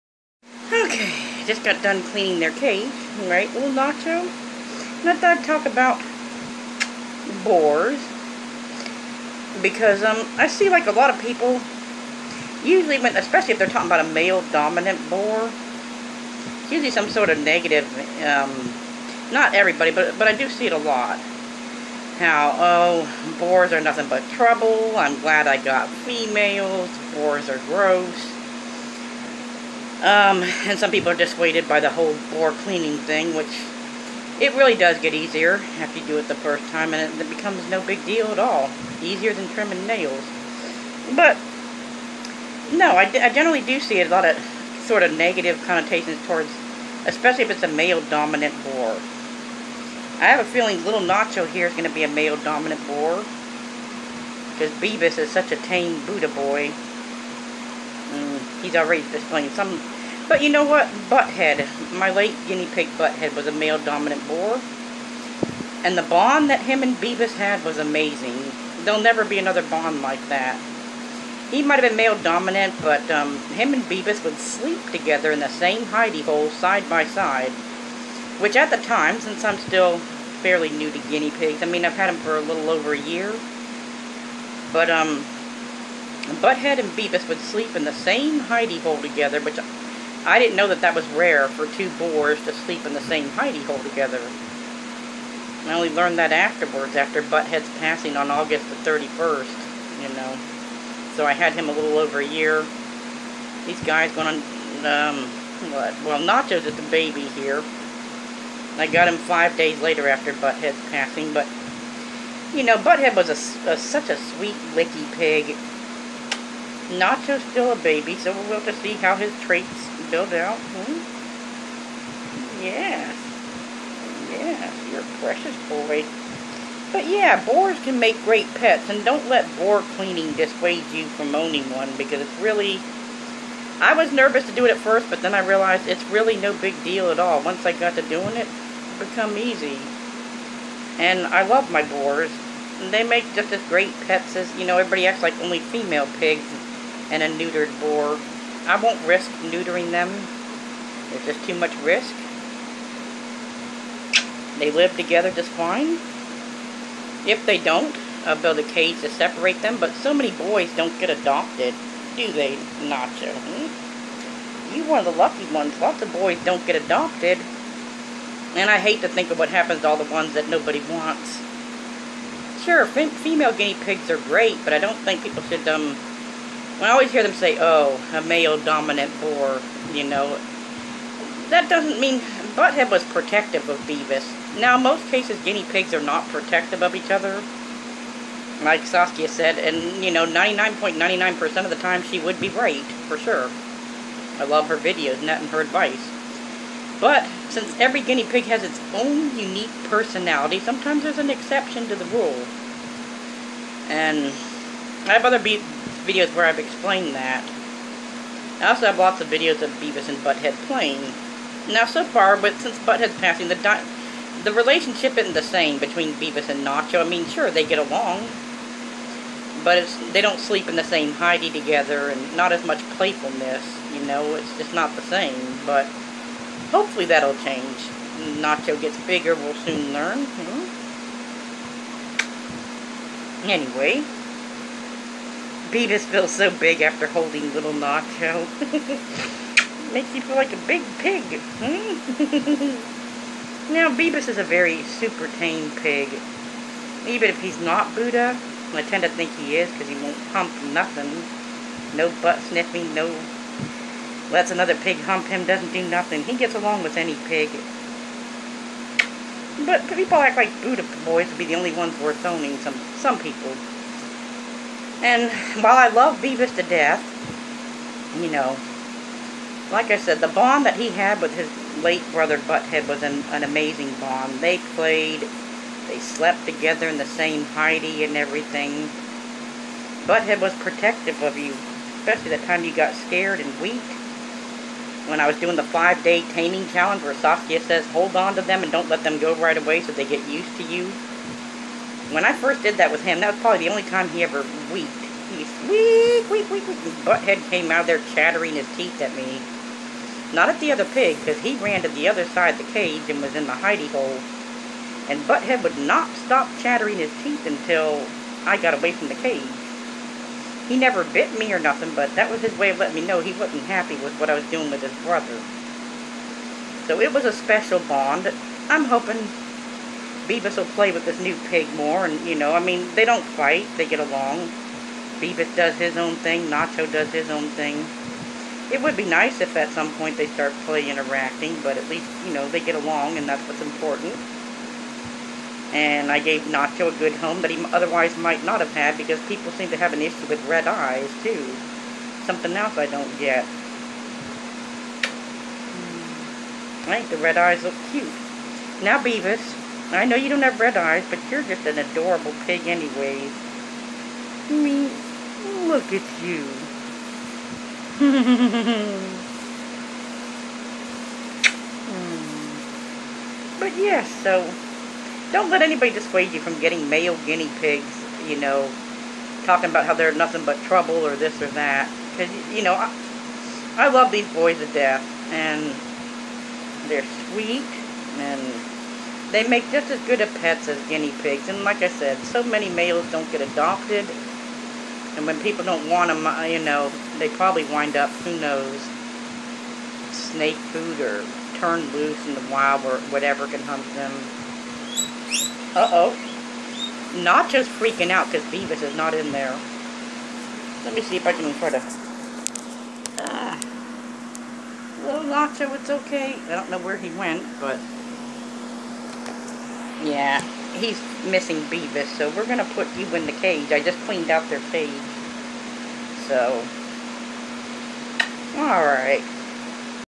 Male Guinea Pigs Make Great Pets Too
I see a lot of people say things like " I would never own any boars, they're gross or male guinea pigs are nothing but trouble "  I'm here to tell you that's just not true and male guinea pigs are as awesome as females.